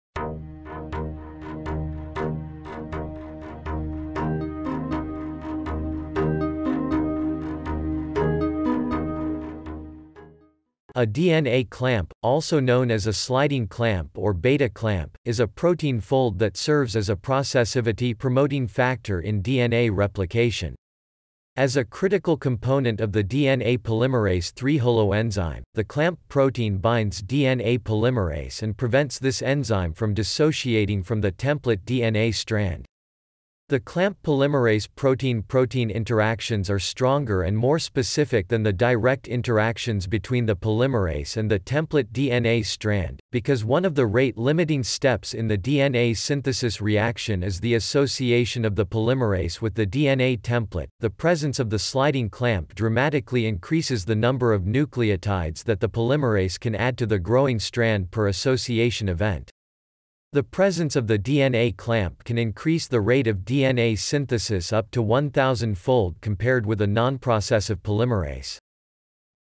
What is DNA clamp? Explain DNA clamp, Define DNA clamp, Meaning of DNA clamp
~~~ DNA clamp ~~~

Title: What is DNA clamp? Explain DNA clamp, Define DNA clamp, Meaning of DNA clamp

Description: A DNA clamp, also known as a sliding clamp or β-clamp, is a protein fold that serves as a processivity-promoting factor in DNA replication. As a critical component of the DNA polymerase III holoenzyme, the clamp protein binds DNA polymerase and prevents this enzyme from dissociating from the template DNA strand. The clamp-polymerase protein–protein interactions are stronger and more specific than the direct interactions between the polymerase and the template DNA strand; because one of the rate-limiting steps in the DNA synthesis reaction is the association of the polymerase with the DNA template, the presence of the sliding clamp dramatically increases the number of nucleotides that the polymerase can add to the growing strand per association event. The presence of the DNA clamp can increase the rate of DNA synthesis up to 1,000-fold compared with a nonprocessive polymerase.